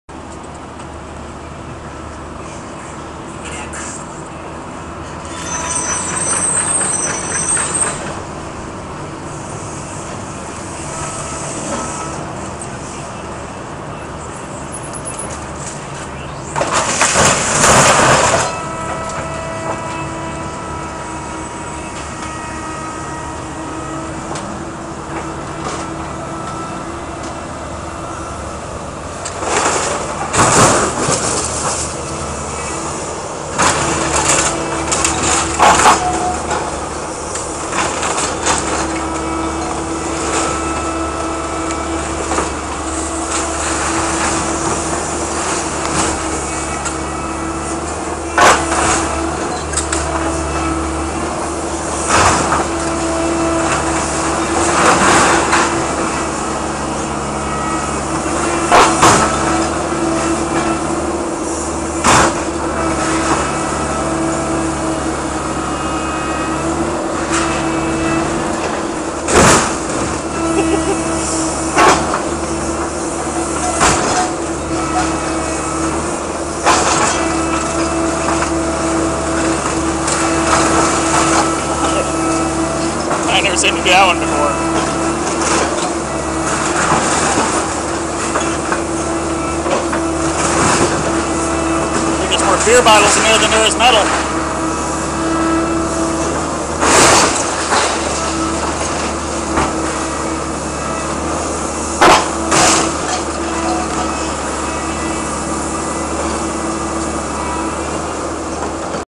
Death of a 80's Ford ranger
Taking a Ford Ranger to the scrap yard, it was minus the motor and beat to a pulp.